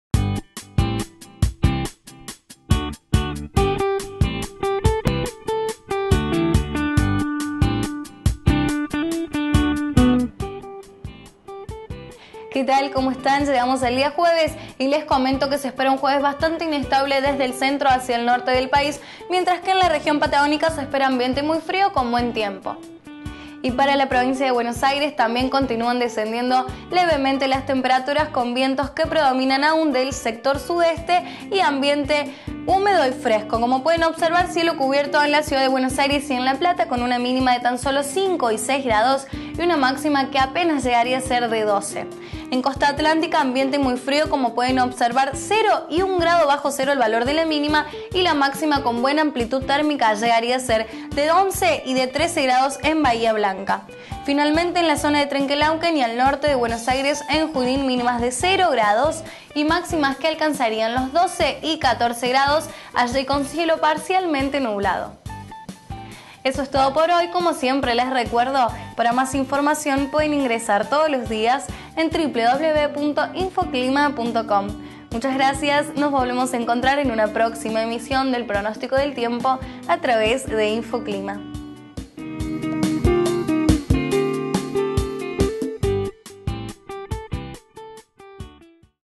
Pronóstico para el 26 de junio de 2014 Buenos Aires - Infoclima TV.
Pronóstico del tiempo elaborado por Infoclima para todo Buenos Aires y la República Argentina. 26 de junio de 2014 - Presenta: Sofía Garone.